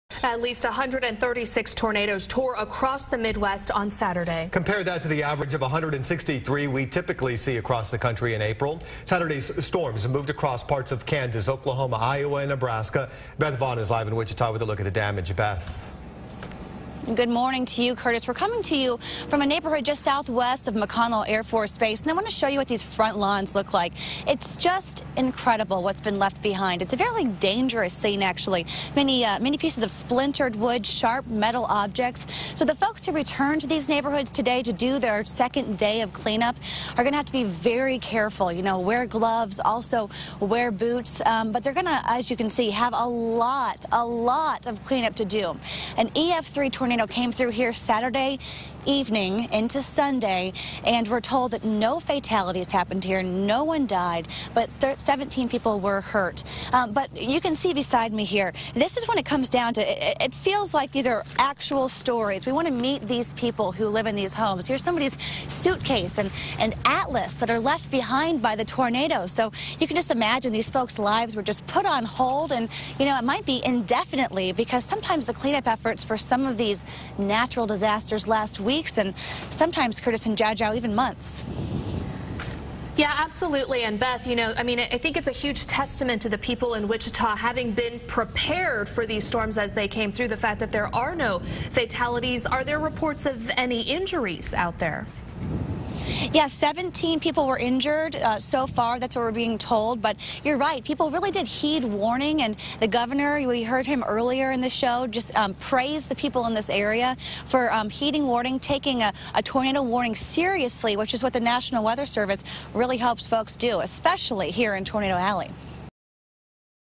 Wichita starts week picking up pieces
Residents in Wichita started the week cleaning up from a weekend tornado that caused widespread damage in the area.